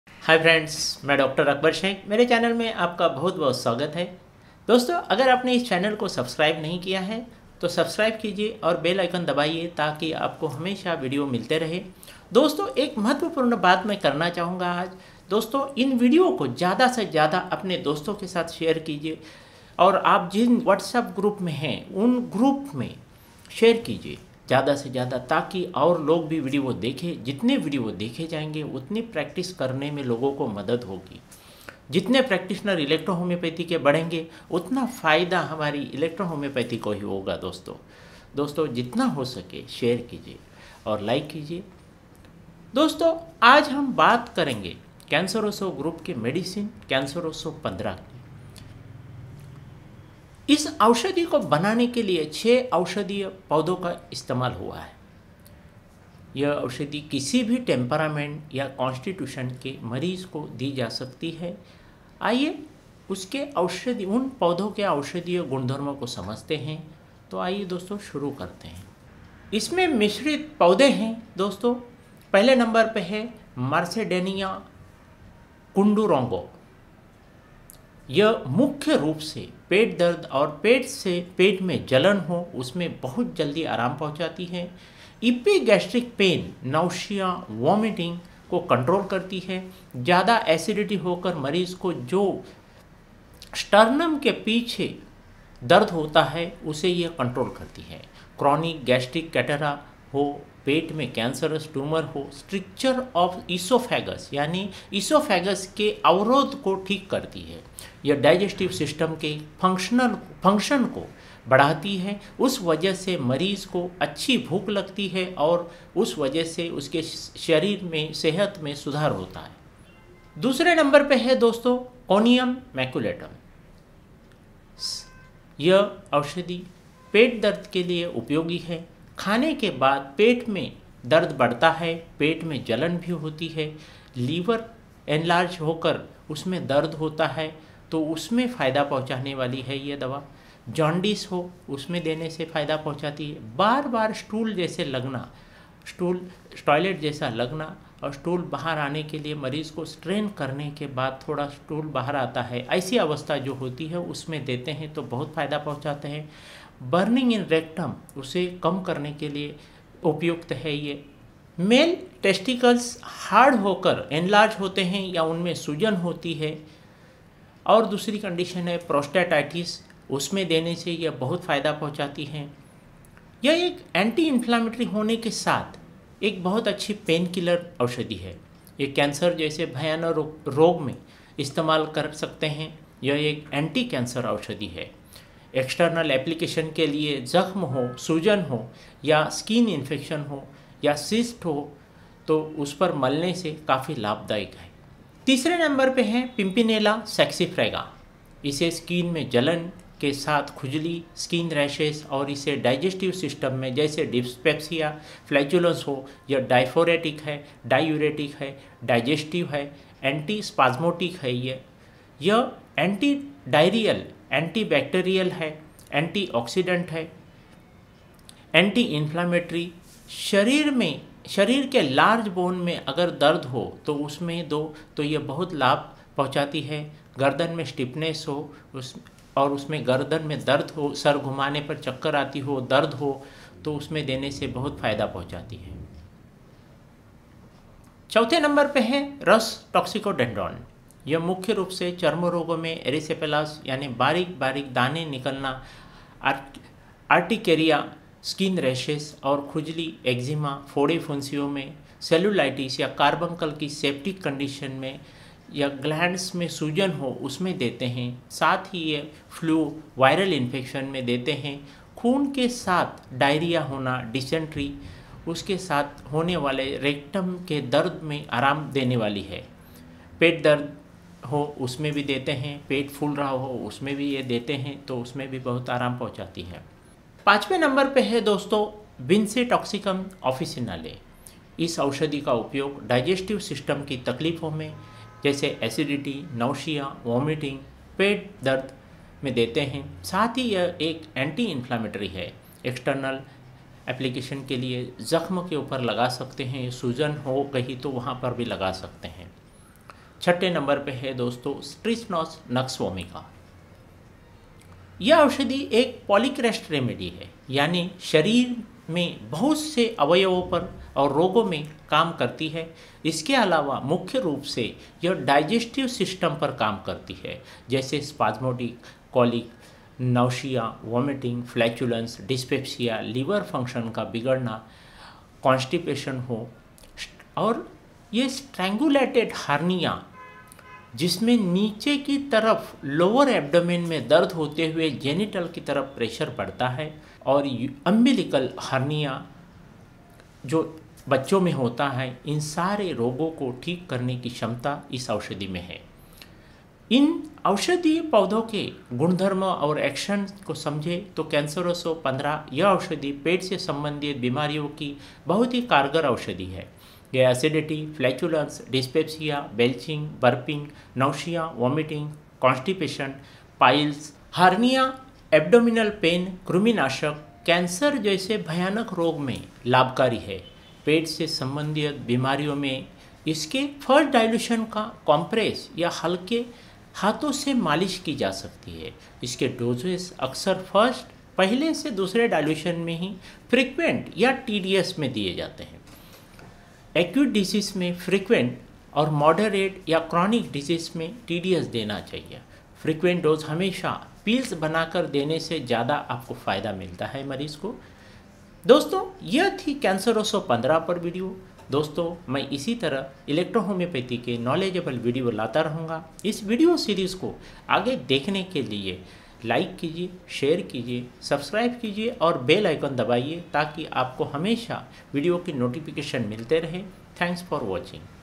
Canceroso 15 electro homeopathy medicine for acidity and Hernia
Canceroso 15 yeh pet se sambandhit beemariya jaise chronic acidity, Flatulence, dyspepsia, Belching, burping, nausea, vomiting, constipation, piles, hernia, abdominal pain, worms, ki electro homeopathy ki bohot acchi medicine hai.